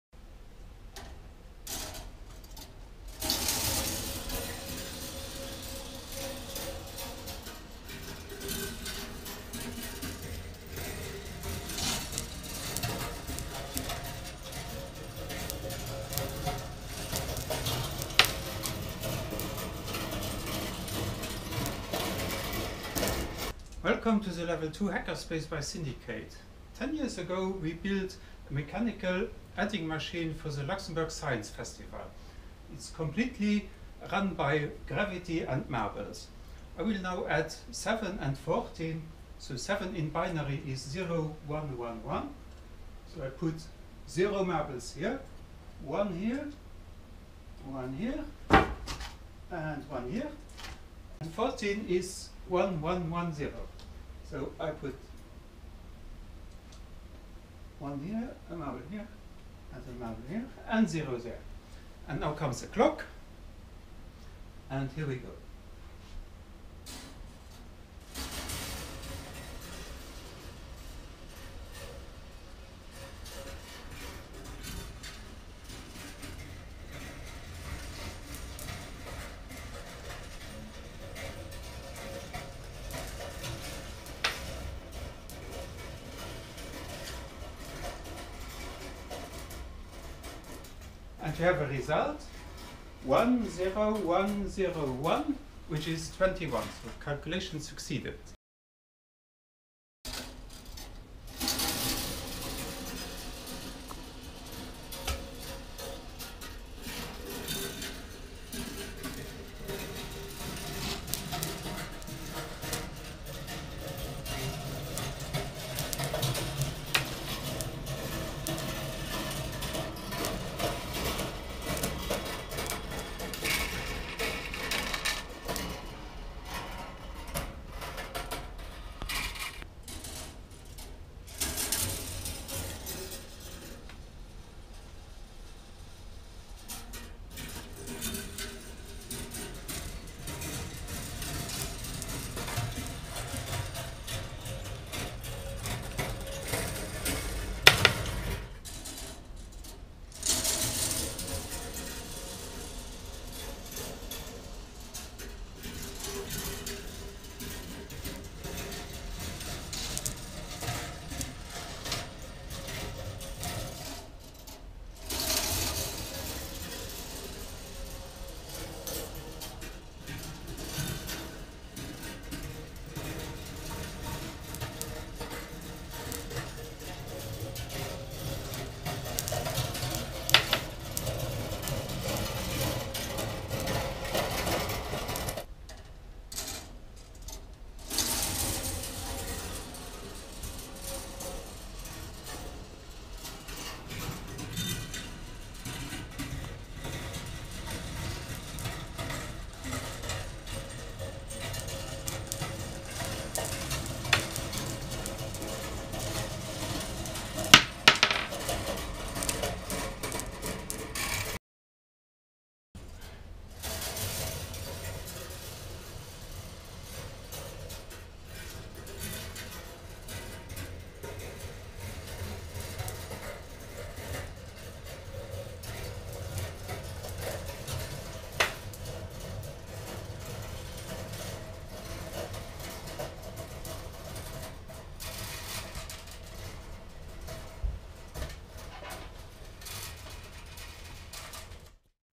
Marbleadder: a marble operated adding machine with true logic gates
Completely built out of copper, this machine can add 2 numbers of 4 bits (between 0 and 15) and visualizes how to combine simple logic gates to execute complex operations.
See my channel for previous marbleadder videos and other topics